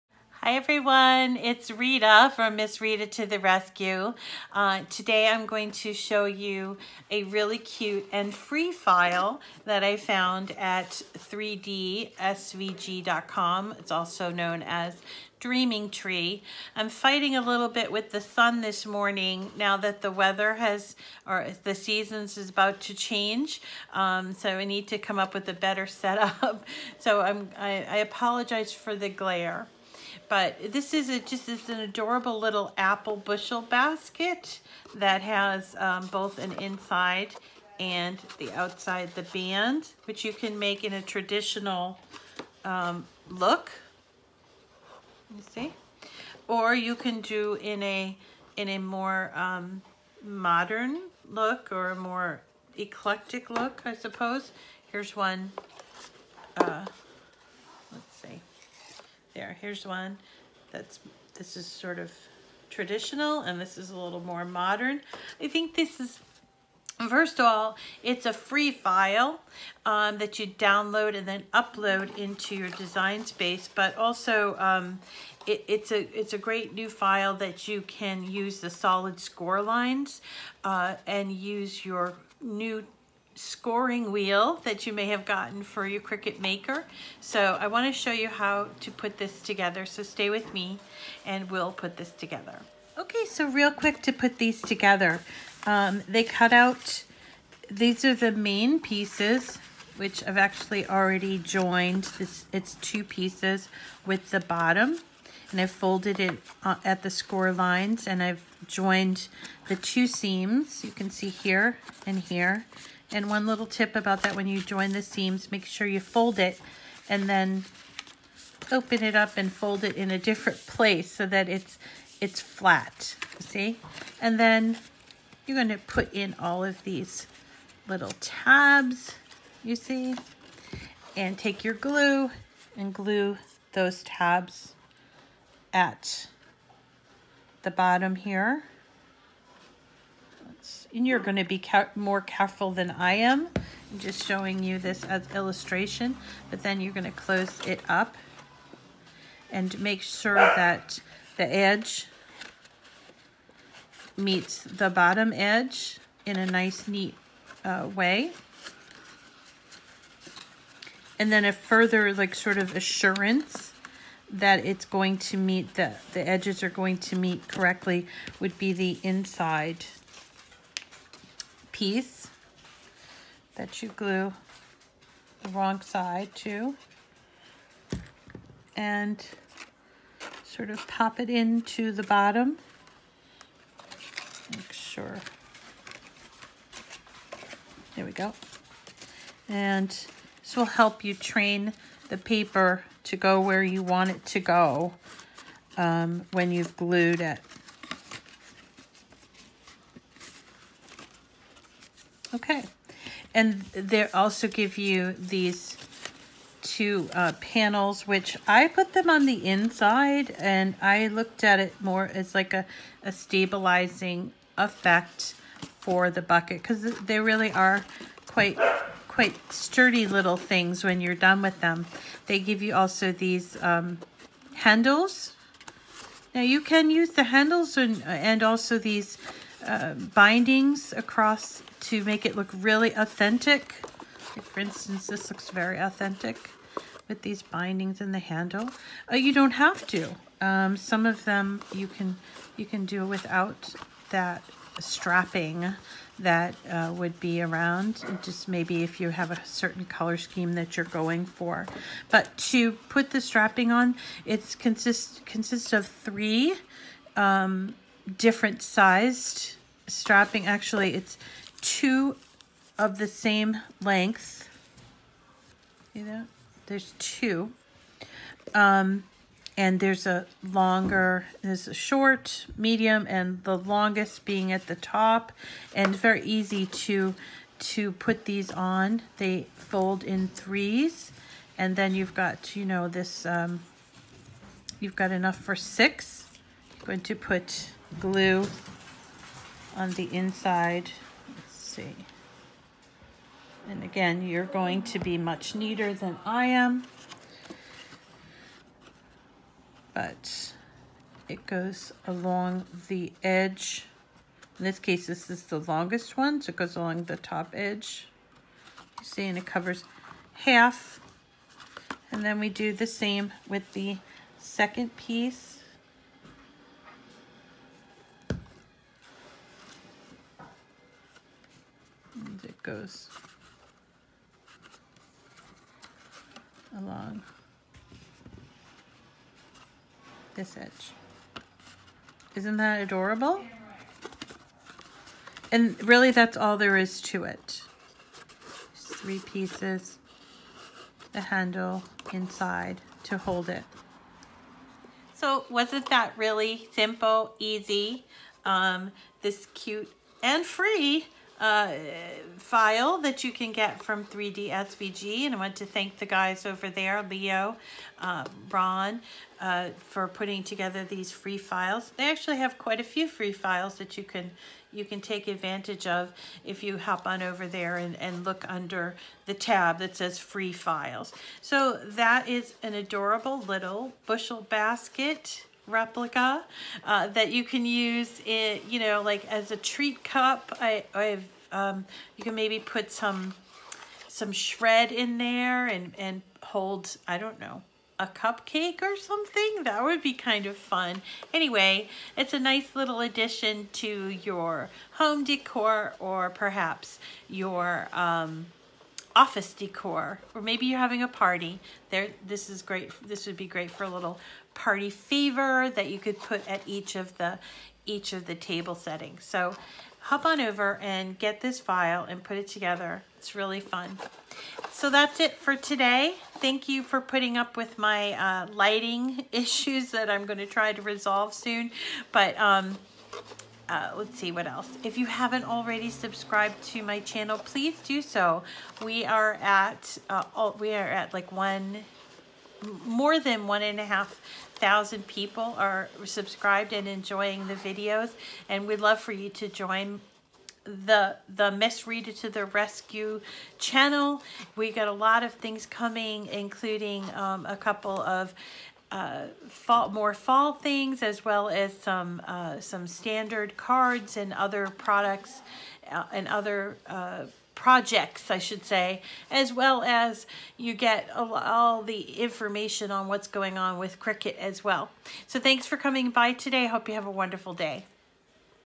Make A Paper Bushel Basket with Your Cricut
Learn how to make a Paper Bushel Basket with your Cricut machine and a free design from Dreaming Tree (3DSVG).

Today I show you how to assemble this adorable paper Bushel Basket with just a few pieces of card stock and your trusty Cricut machine from a FREE file you can get at Dreaming Tree.  This is a perfect little project to practice using your new Scoring Wheel for your Cricut Maker!  Or--if you don't have one yet--you can always use the dashed cut lines that are a part of this file, too.

I, of course, use my Cricut Maker and Cricut Design Space software for ALL of my projects.

If you are in the market for a Cricut Machine or perhaps need to get supplies, please know that I am self supporting and I make money when you use my link: